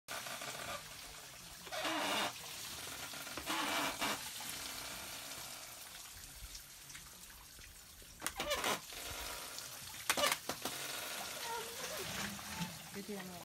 Extracting agave juice for fermentation
This is a process unique to Mezcal Campanilla in San Luis Potosi. Cooked agave fibers are placed in the "hammock" and then squeezed to ring out juice.